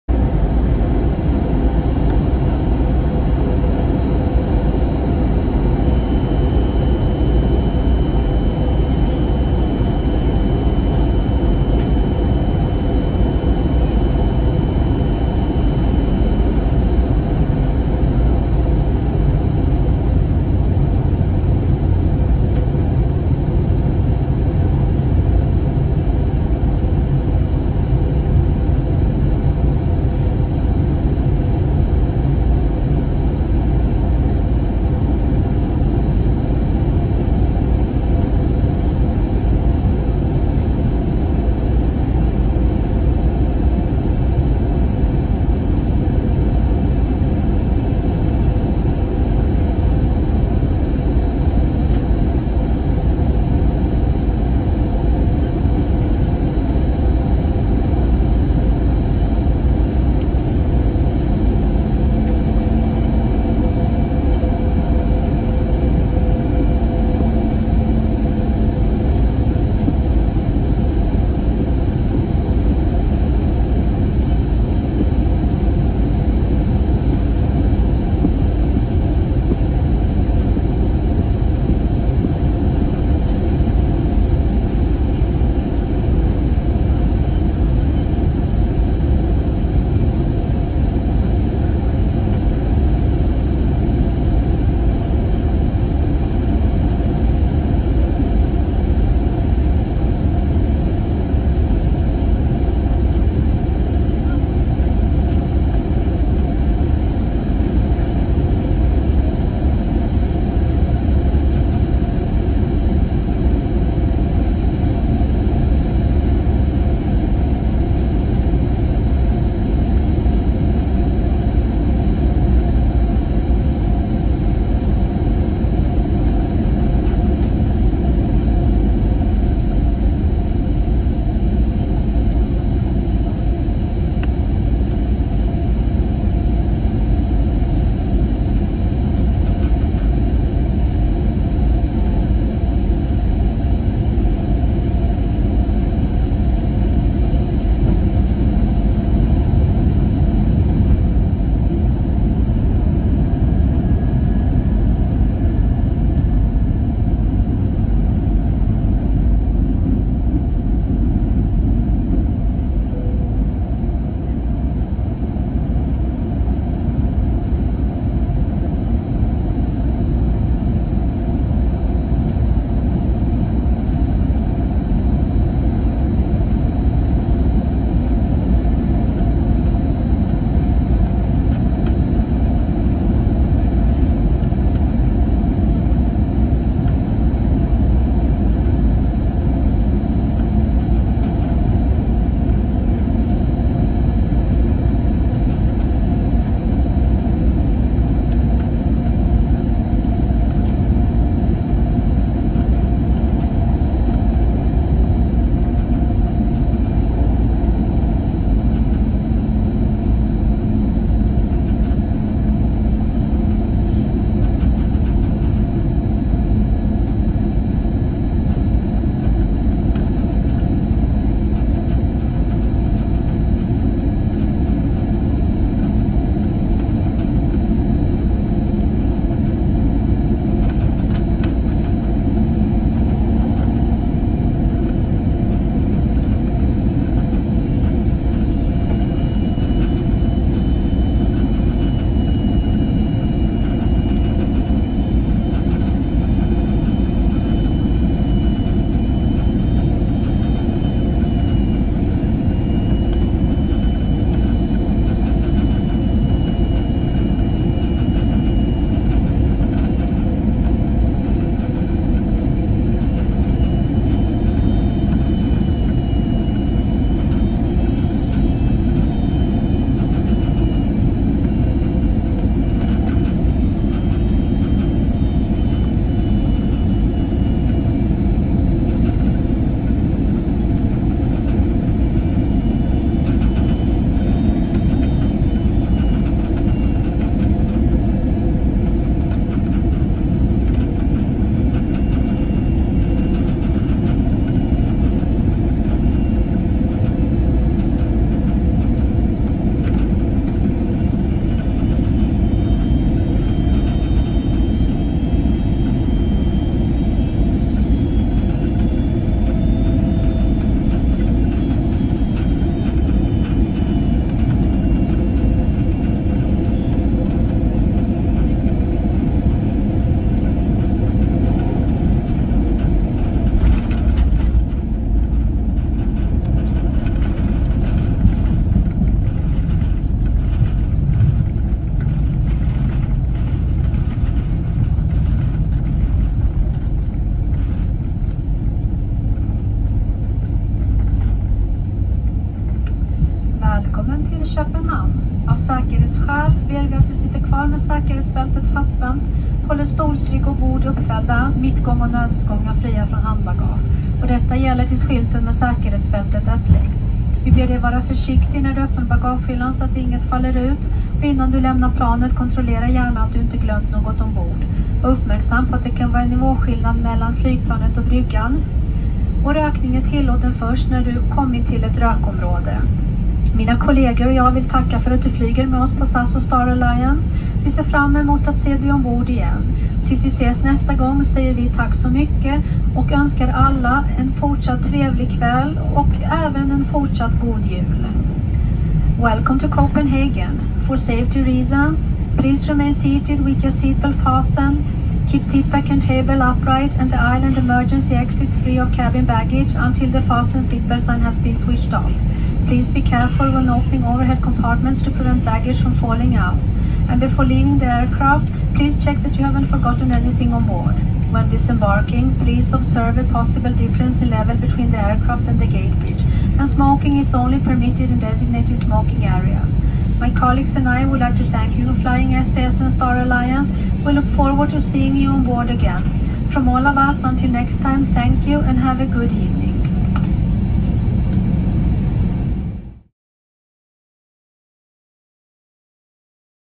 SAS A320neo | Stockholm to Copenhagen Landing
Origin: Stockholm [ARN/ESSA]
Destination: Copenhagen [CPH/EKCH]
Duration: 0.51
Date: December 25, 2018
Flight number: SK 1429
Seat: 1A
Cabin: SAS Plus
Registration: SE-ROC
Airplane type: A320neo